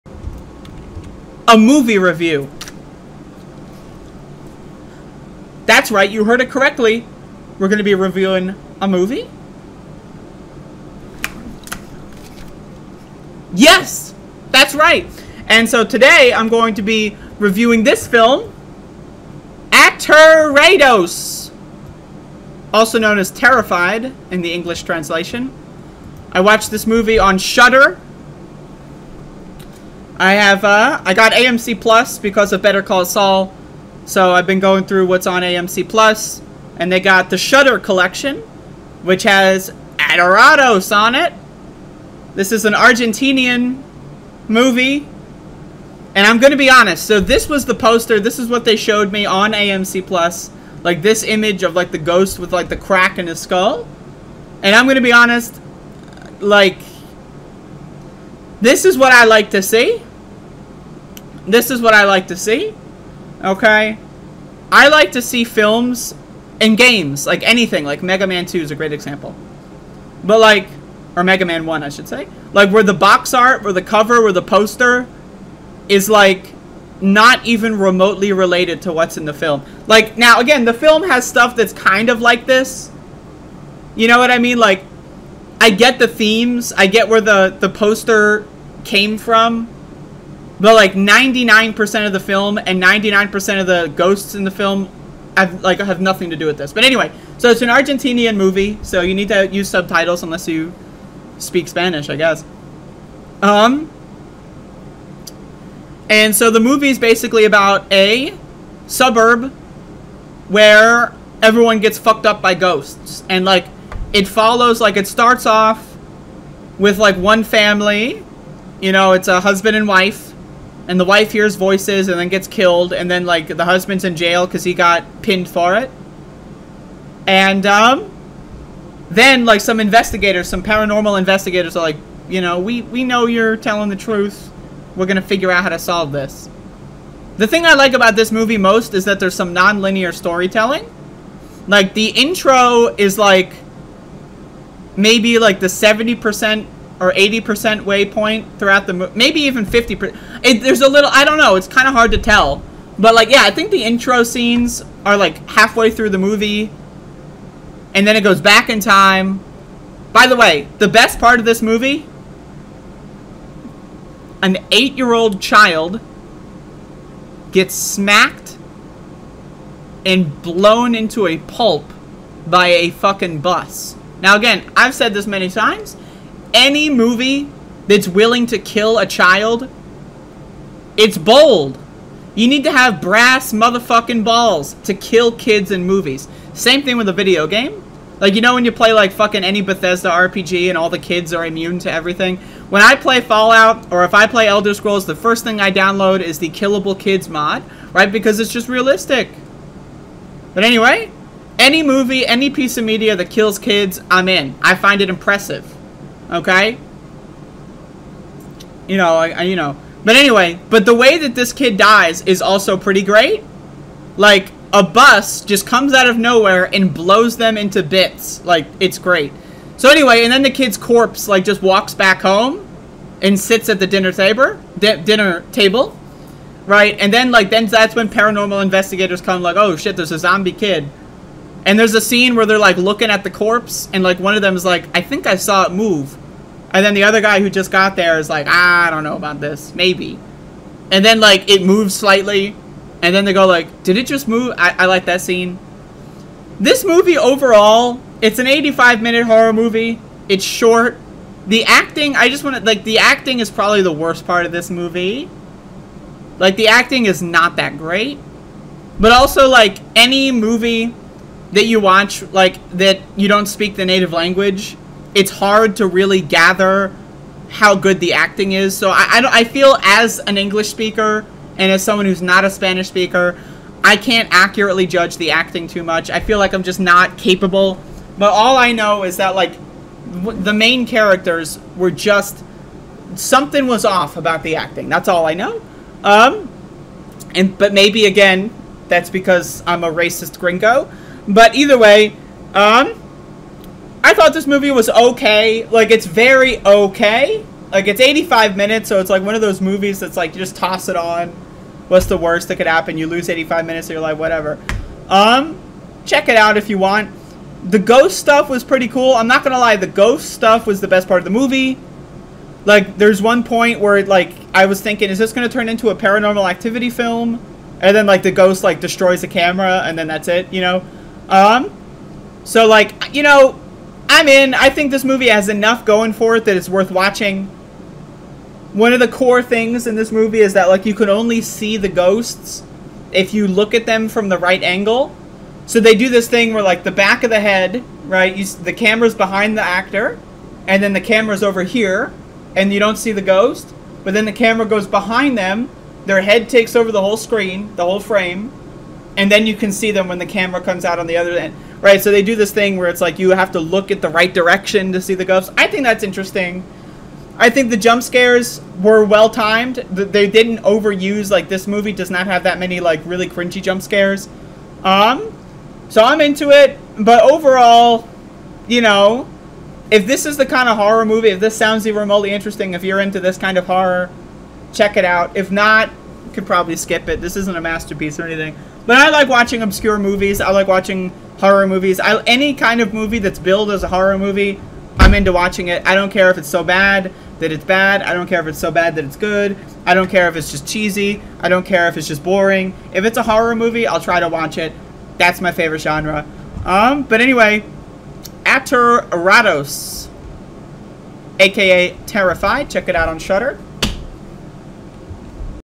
Aterrados (Terrified) (2017) Movie Review - Decent Ghost Hunting Movie That Doesn't Last Too Long
Send me money to help me eat tomorrow!